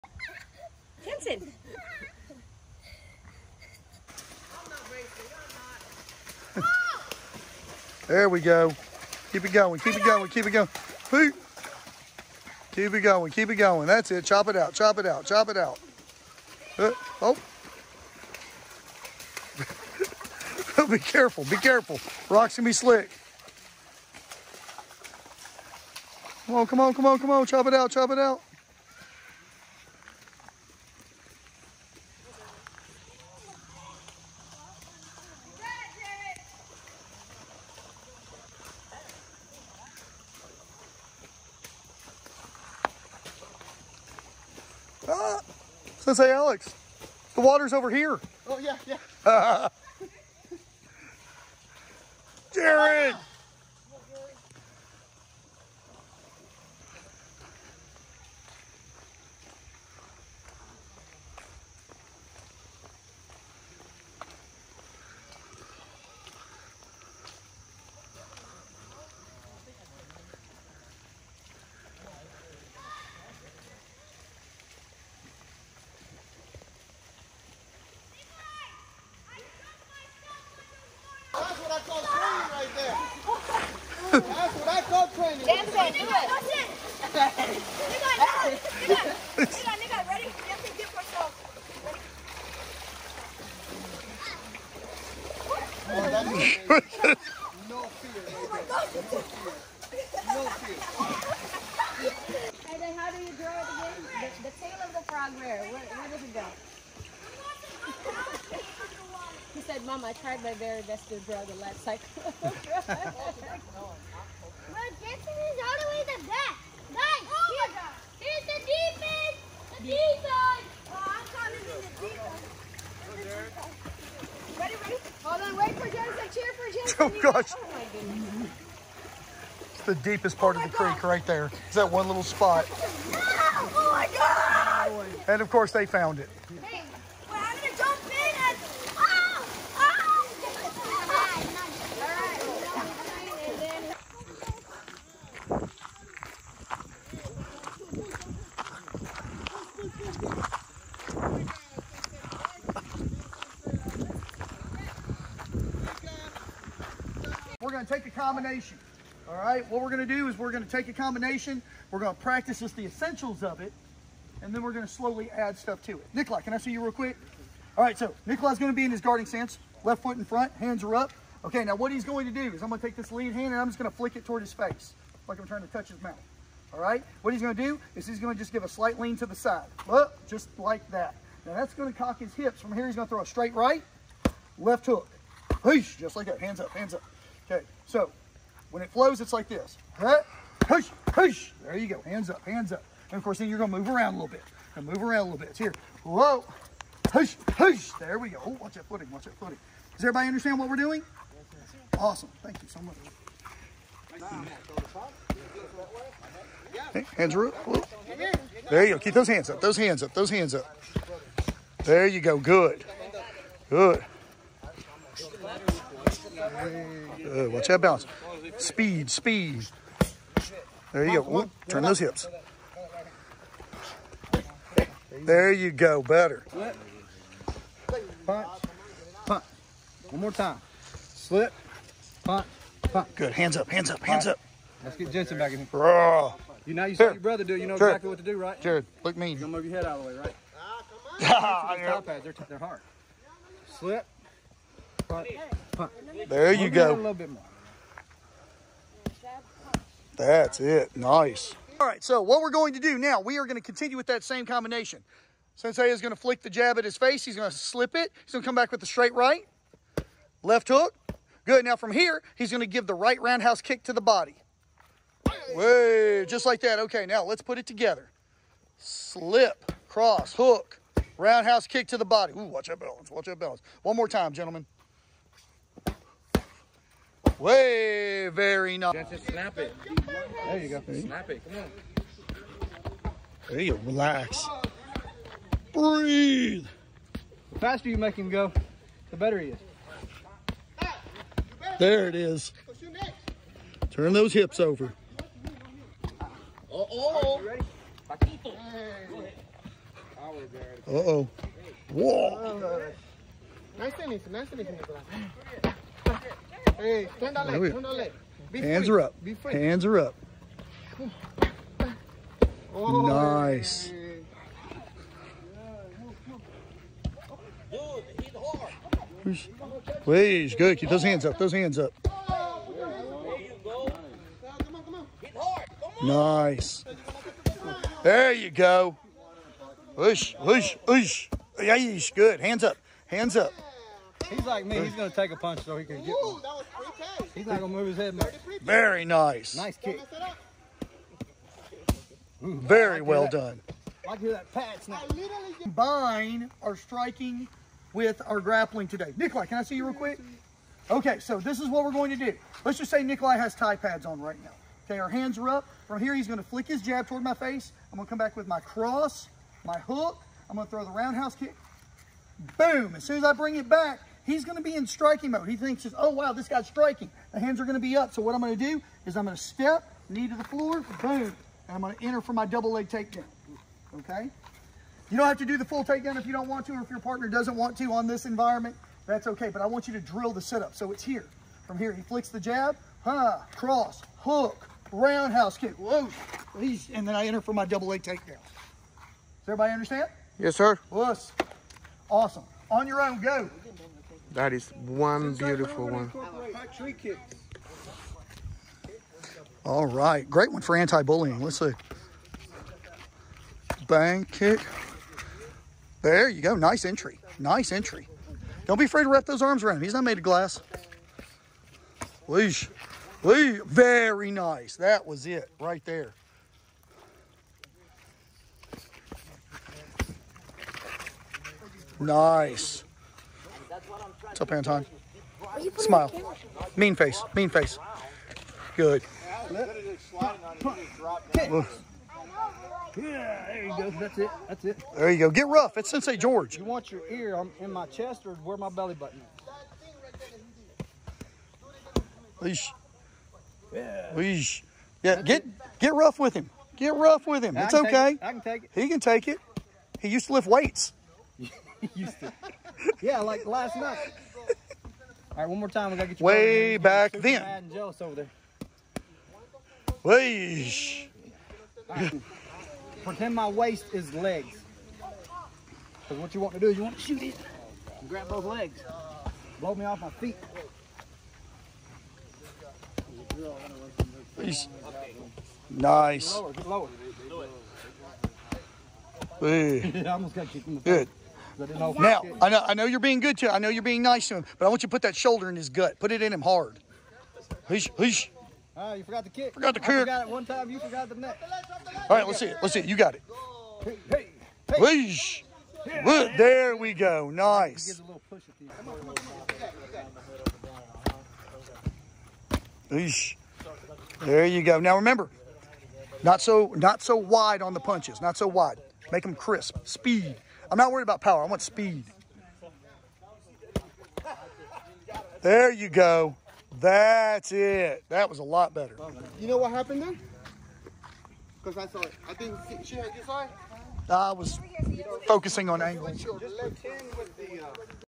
More nature training! Young warriors practicing in the wilderness! Caps ninjas!
A fun day of training our self defense skills in nature.